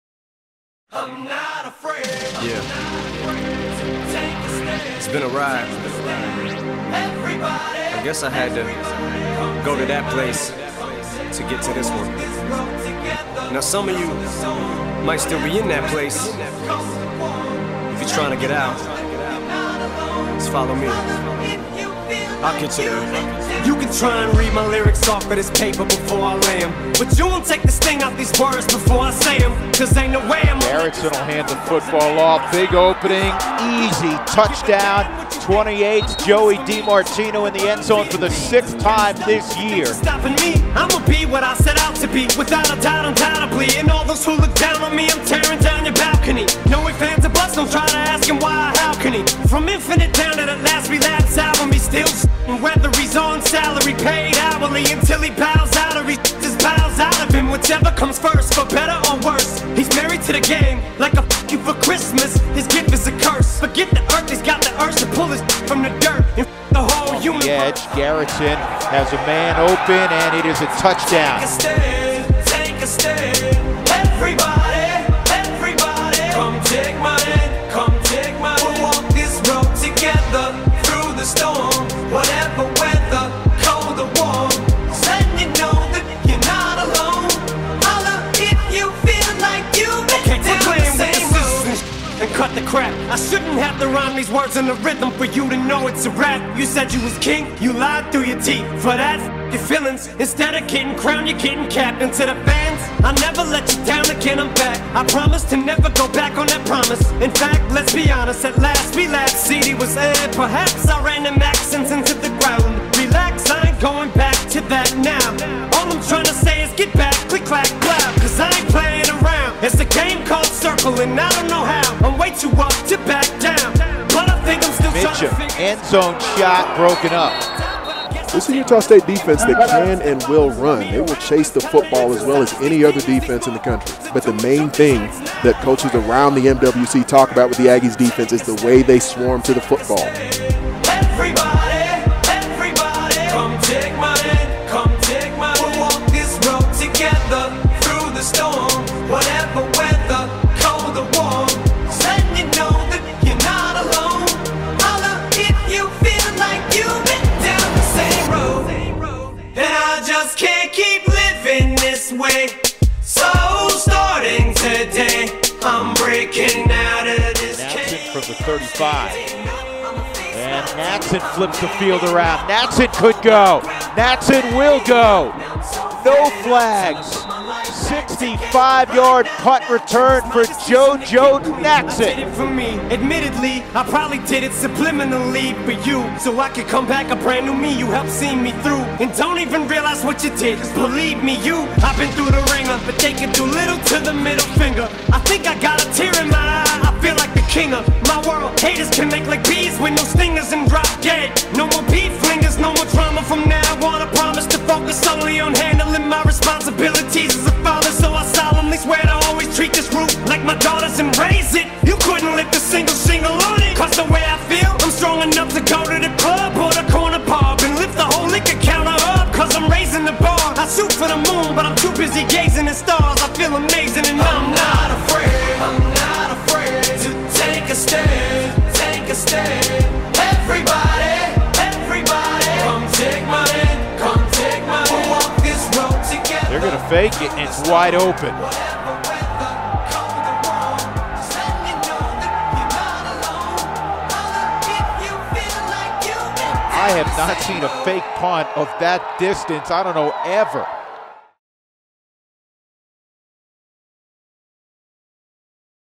2013 USU vs New Mexico Highlight
A great win for the Utah State Aggies over New Mexico.